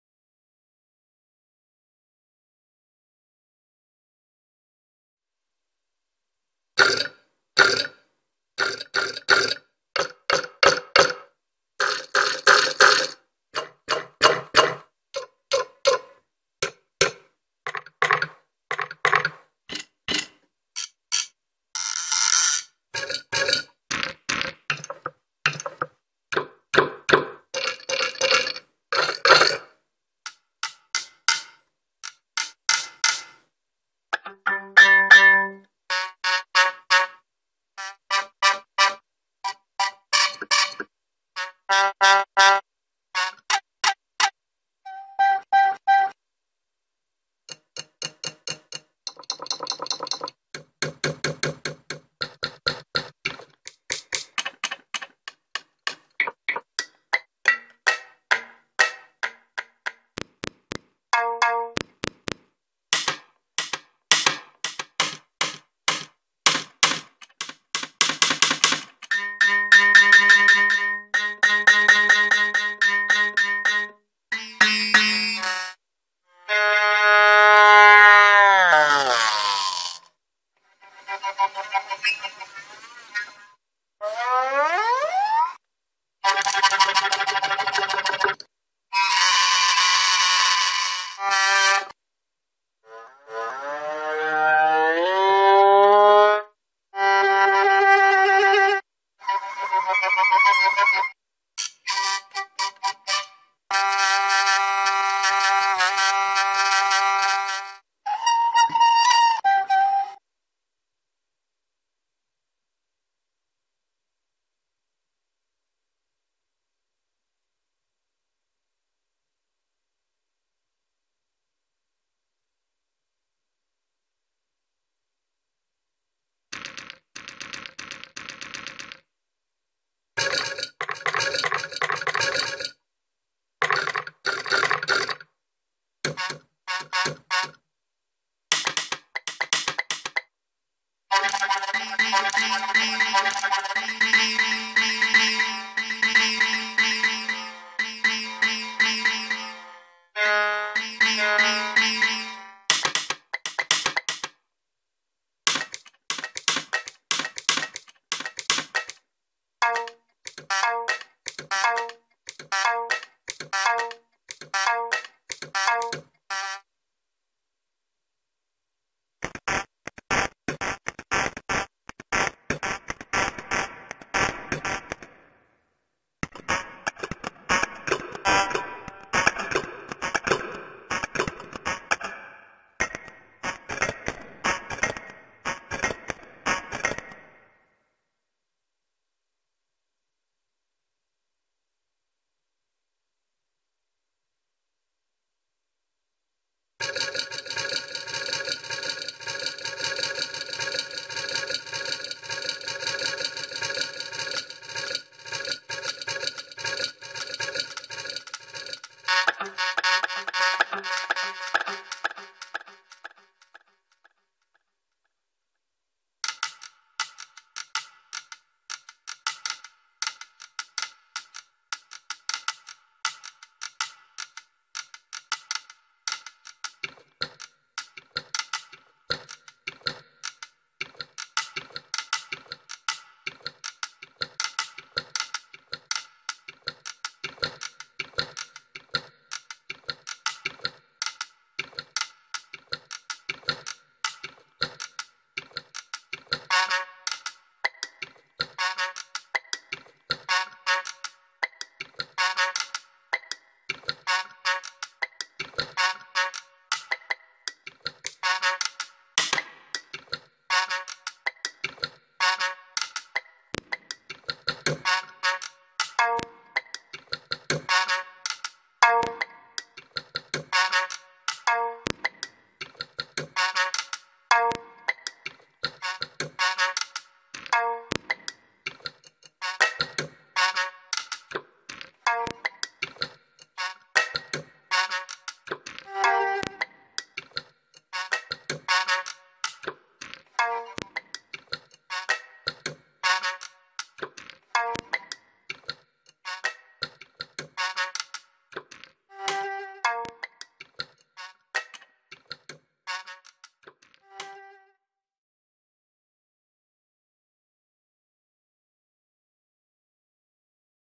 BuzzDOG - Percussive Patch DEMO
In this video, I'm going through the sounds of a "Percussive" patch of the "Lira Korbowa II - BuzzDOG" library for Kontakt.
In this short presentation, the sounds, few effects, step sequencer and arpeggiator are being used.

The full library is entirely derived from hurdy-gurdy sounds and includes noises, squeaks, hits and a whimsical mixture of other unholy occurrences in and on the instrument ;)

The full library includes HOST SYNCED patches of hurdy-gurdy trompette in a variety of time signatures and tempi.

Piotr Nowotnik